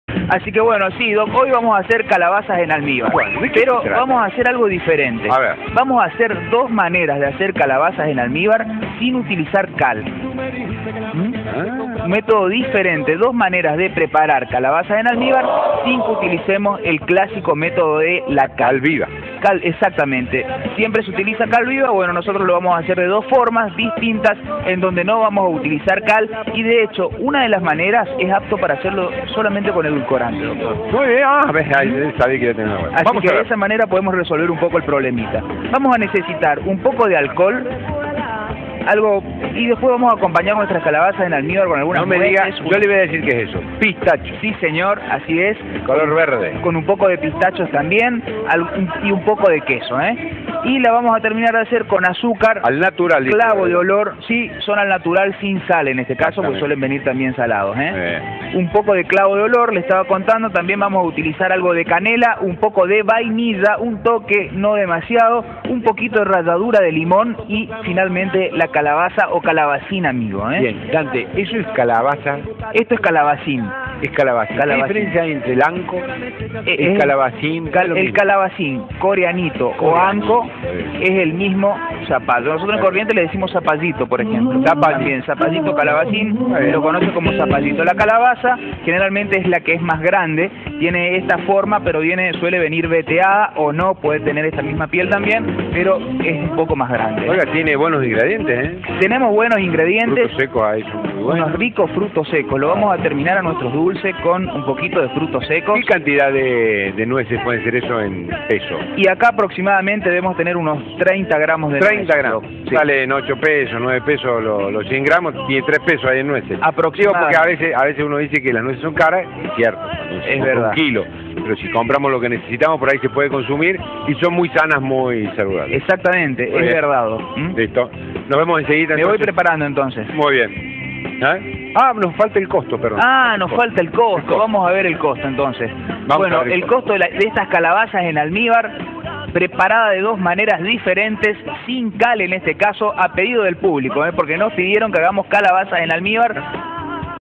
calabaza en almibar pt1 20110401.3gp
Hoy Dante nos enseña a preparar dos maneras distintas de calabazas en almíbar, sin el método de la cal.